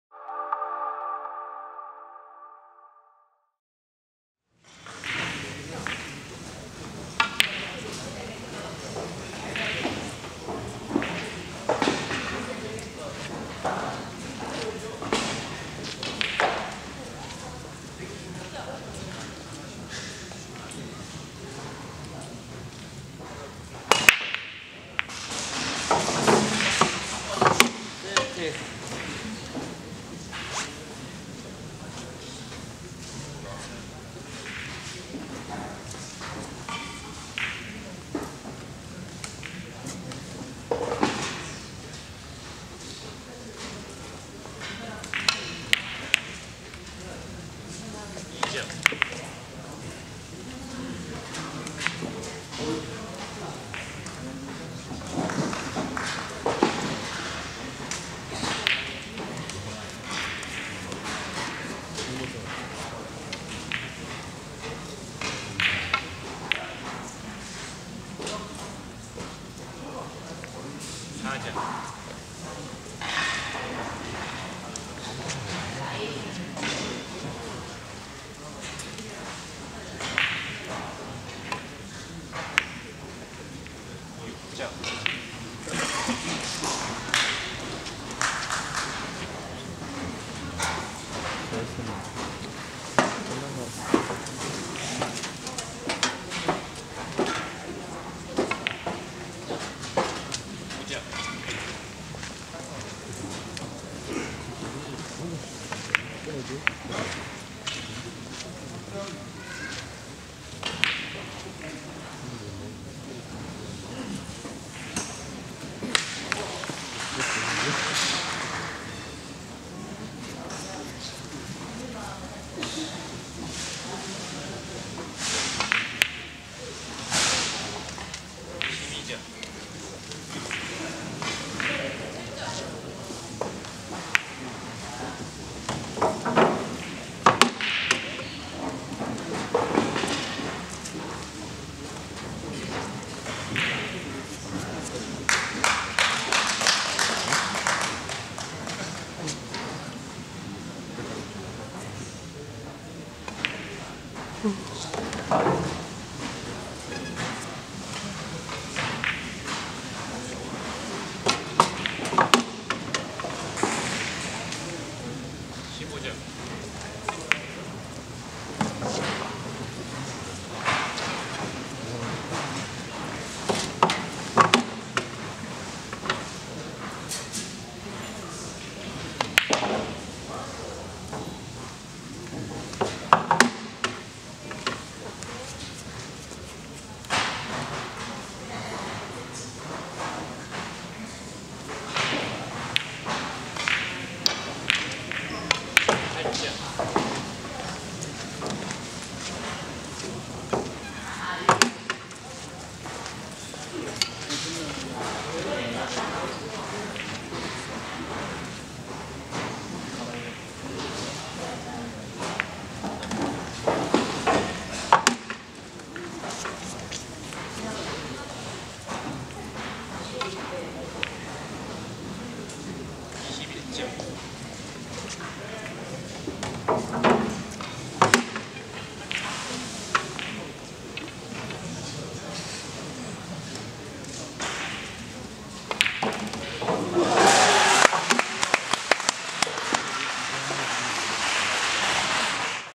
포켓9볼 준준결승 15 이귀영 vs 김정현 POOL 9 Ball Quarter-fianl Rack 15
Pool 9 ball Quarter-fianl rack15
2013 인천 전국체전 포켓9볼 여자일반부  준결승전 이귀영 VS 김정현 랙 15
2013 Incheon national sports festival billiard